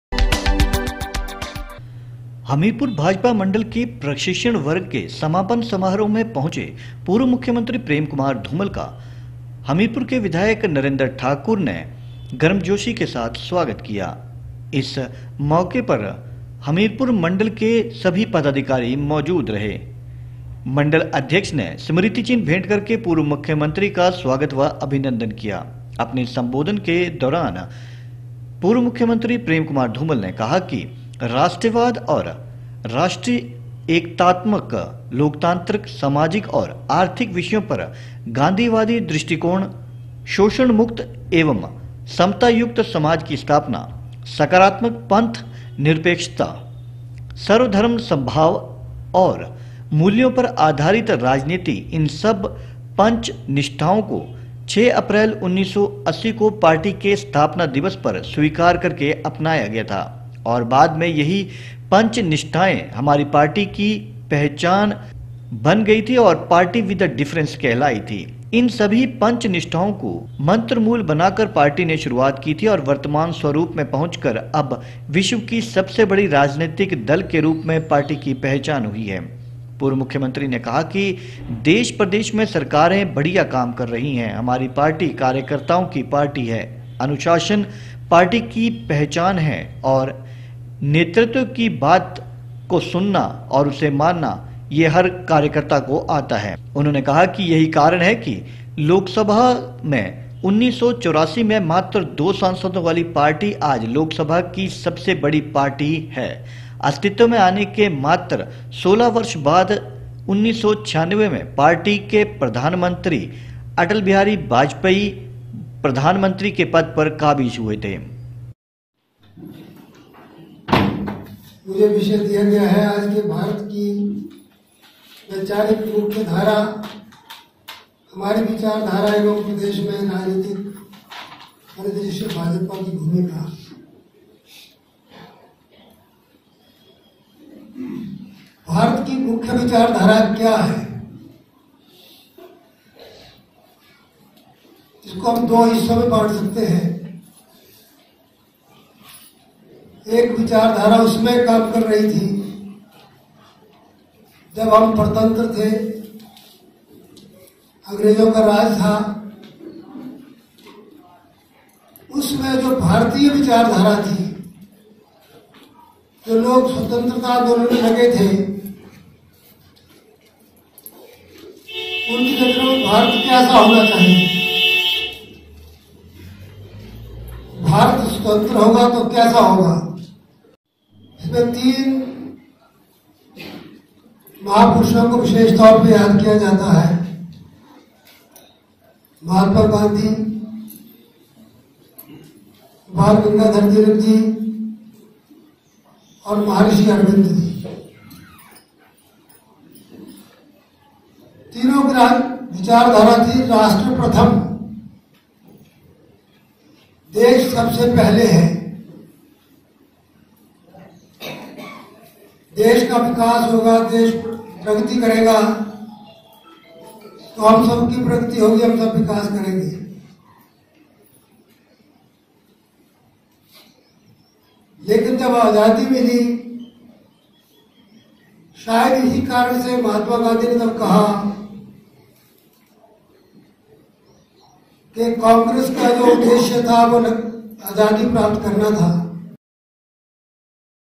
प्रो धूमल ने कहा देश प्रदेश में सरकारें बढ़िया काम कर रही हैं हमारी पार्टी कार्यकर्ताओं की पार्टी है
प्रो० धूमल ने कहा कि देश प्रदेश में सरकारें बढ़िया काम कर रही हैं, हमारी पार्टी कार्यकर्ताओं की पार्टी है। अनुशासन हमारी पहचान है